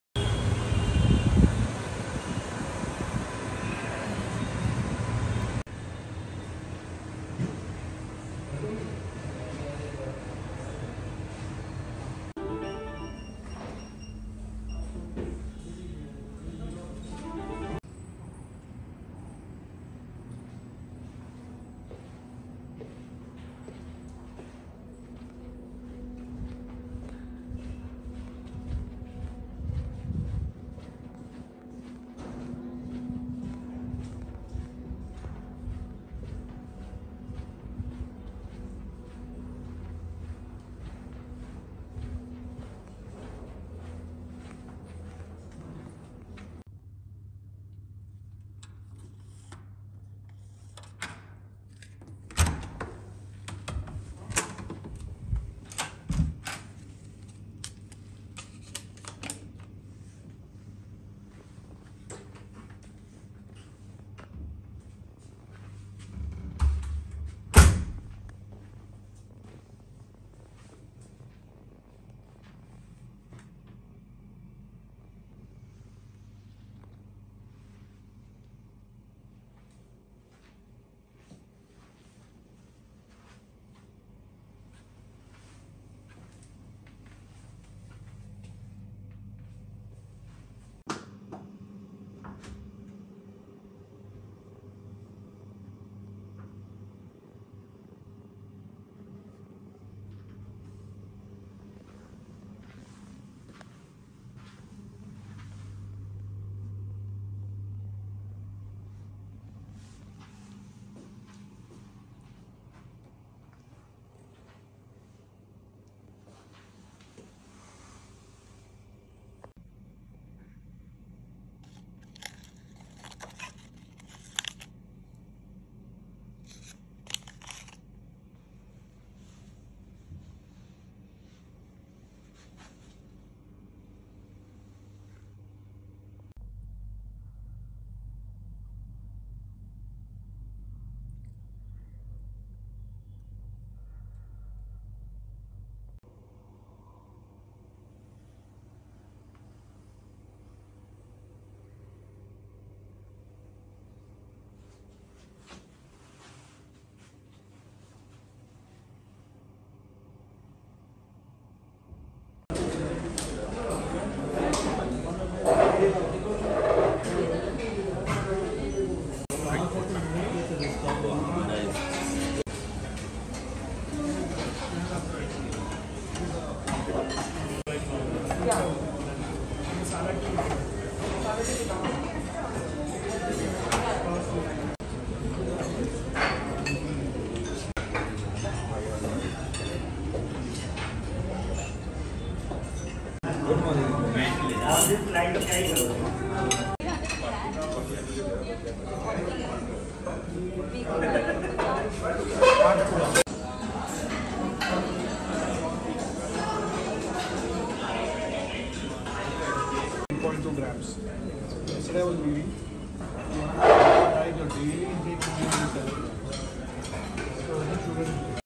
Inside Holiday Inn Express Hyderabad, Banjara Hills, India - Room, Breakfast - Quick Tour
This hotel is located in a strategic location in the upscale area of Banjara Hills. It is walking distance to the GVK Shopping Mall. Breakfast is complimentary and room is clean.